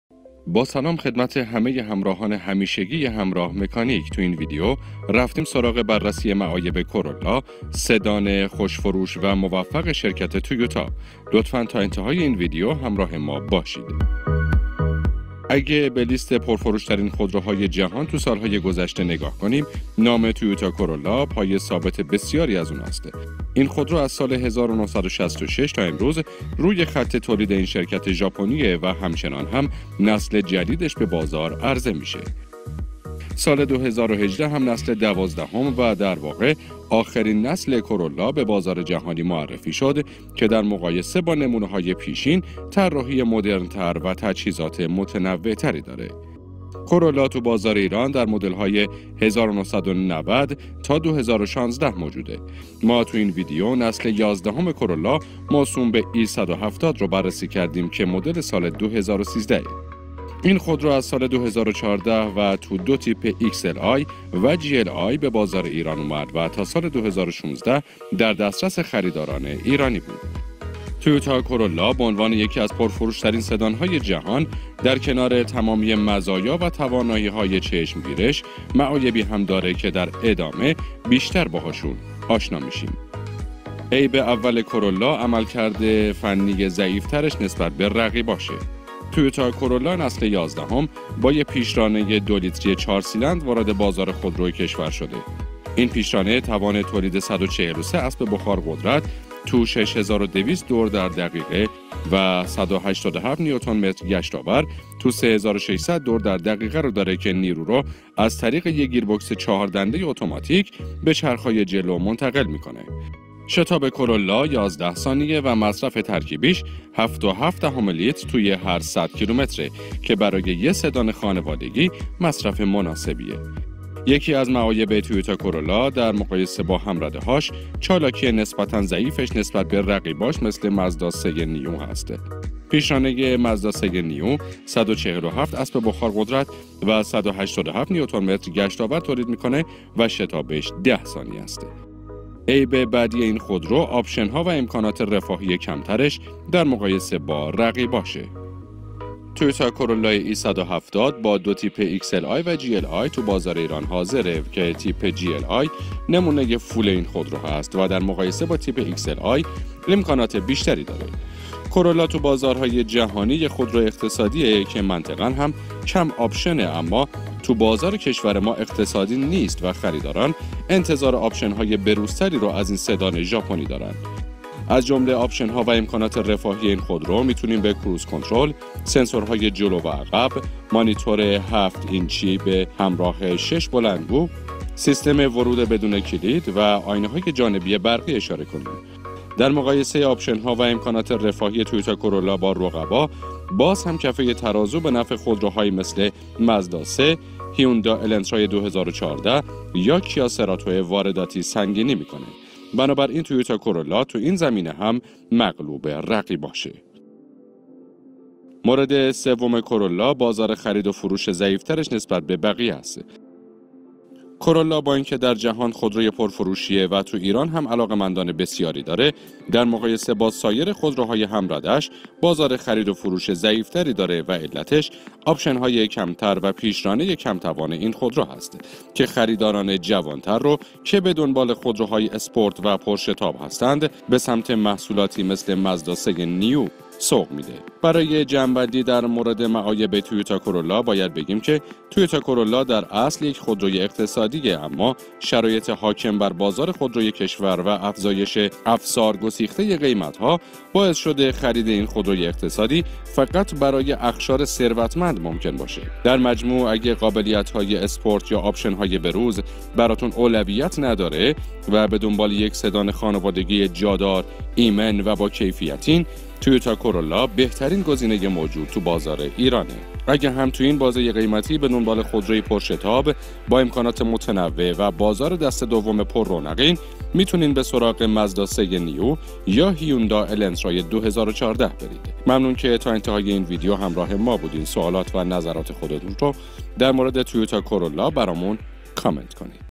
با معایب تویوتا کرولا آشنا شوید
تویوتا کرولا خودروی با کیفیت ژاپنی است که یکی از پرفروشترین خودروهای جهان هم به حساب می‌آید، تو این کلیپ قصد داریم به معایب این خودرو بپردازیم
شما چه نظری در مورد این خودرو دارید